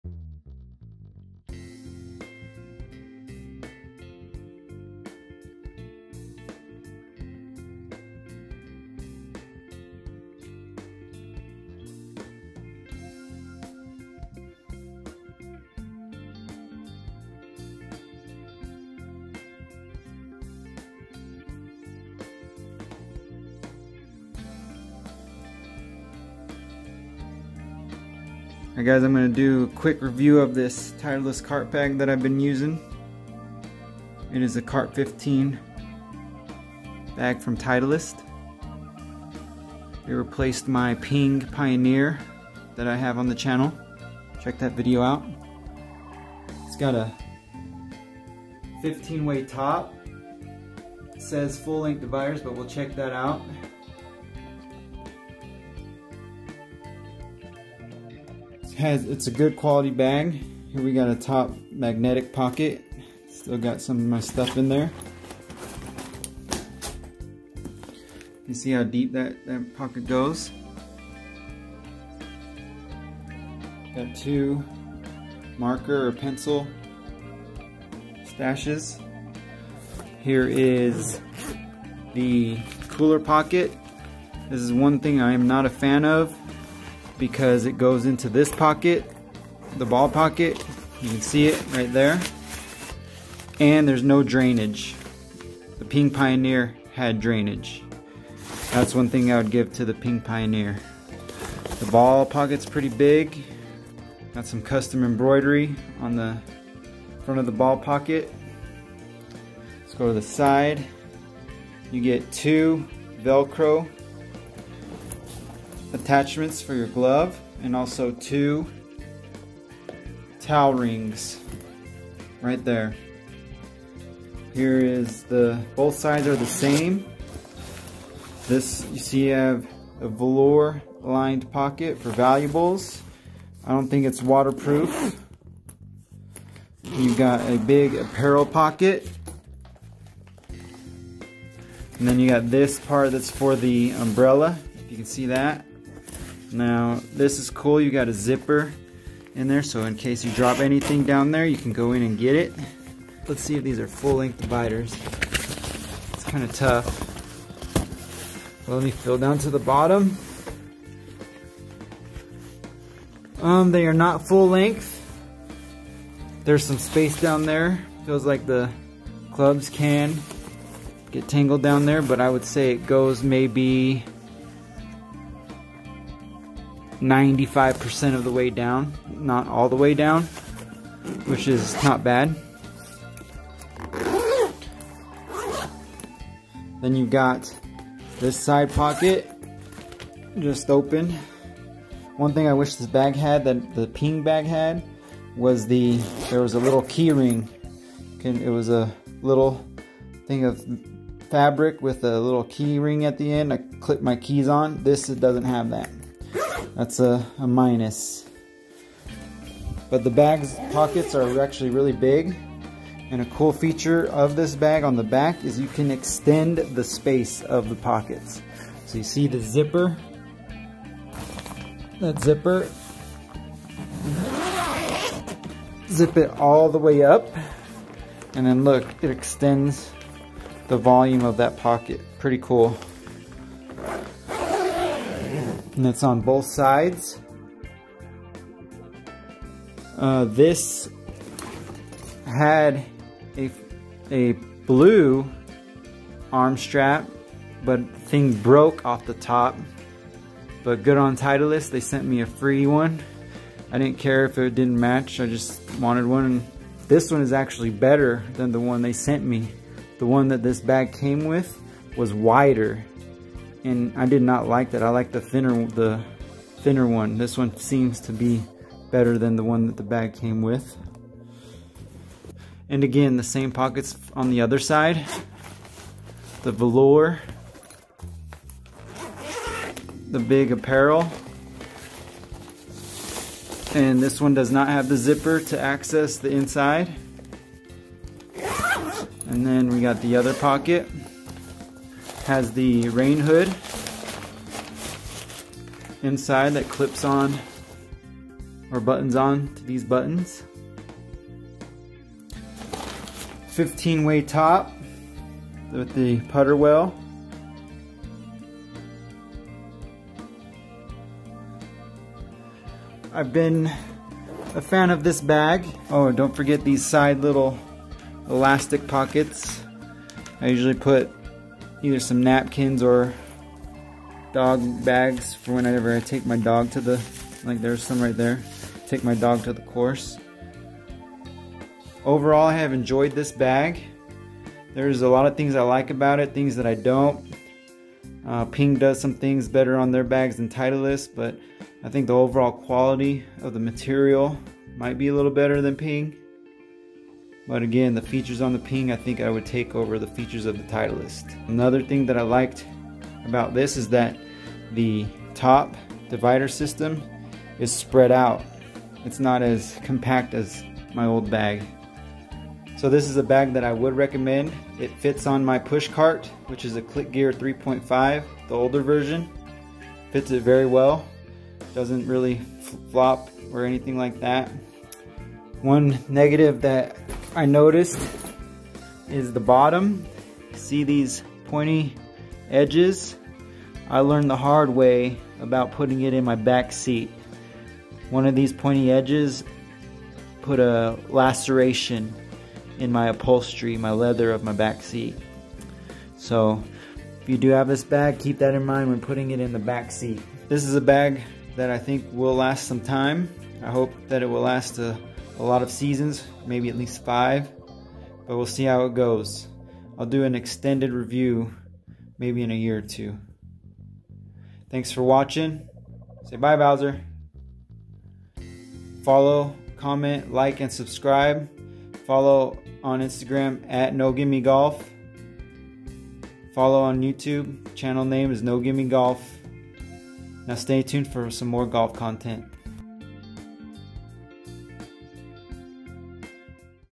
Golf Bag Review. 2022 Titleist Cart 15 Bag. Is this better than my old Ping Pioneer cart bag?
I have a golf bag review for everyone. It is the Titleist Cart 15 bag. It is a great Cart bag, but it has some flaws. It replaced my old Ping Pioneer cart bag. Does it beat the Ping? Check out the video to see.